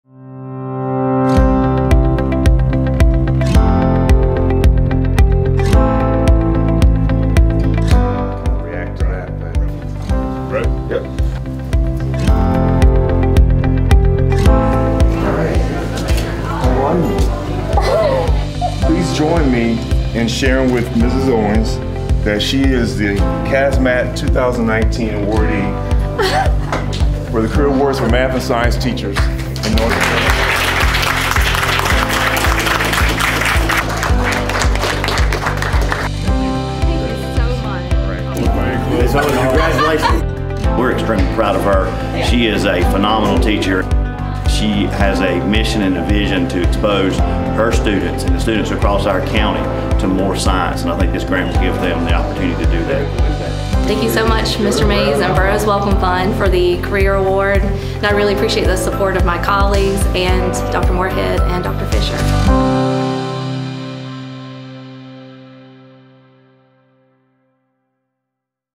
Beverly Owens, Kings Mountain Middle - 2019 Career Awards for Science and Mathematics Teachers
Burroughs Wellcome Fund Rewards Outstanding STEM Teachers in North Carolina

RESEARCH TRIANGLE PARK, NC – During the past two weeks, Burroughs Wellcome Fund Program Officer Alfred Mays collected miles on North Carolina highways.  From the Fund’s headquarters in Research Triangle Park, Mays traveled west to King’s Mountain then east to Ansonville. He then visited Battleboro in Edgecombe County.  And finally, it was back west to West Marion and Yancey in the Blue Ridge.  All this driving was to personally inform the 2019 recipients of the Fund’s Career Award for Science and Mathematics Teachers, which recognizes outstanding STEM classroom teachers in North Carolina public schools.

This year’s recipients are:

Casandra Cherry
Philips Middle School
Edgecombe County Schools

Renata Sabine Crawley
West Marion Elementary
McDowell County Schools

C. Justin Jones
Ansonville Elementary School
Anson County Schools

Beverly Owens
Kings Mountain Middle School
Cleveland County Schools

Sara-Elizabeth (Sallie) Senseney
Mountain Heritage High School
Yancey County Schools

“Witnessing the reaction of the teachers and their students is something I’ll never forget,” said Mays.  “This award recognizes the excellence of an individual teacher, and will impact the entire school and school district.”

The Career Award for Science and Mathematics Teachers was created in 2009 to provide outstanding STEM teachers in NC with professional development, equipment and supplies, and salary support.

“Teachers are in a unique position to make a difference in a child’s life,” said Fund President Dr. John Burris.  “We awarded individuals who have an infectious enthusiasm for the STEM subjects and teach with empathy and skill.”

In the decade since the creation of the Career Awards for Science and Mathematics Teachers, the Fund has recognized 33 N.C. teachers for an investment of more than $5.4 million.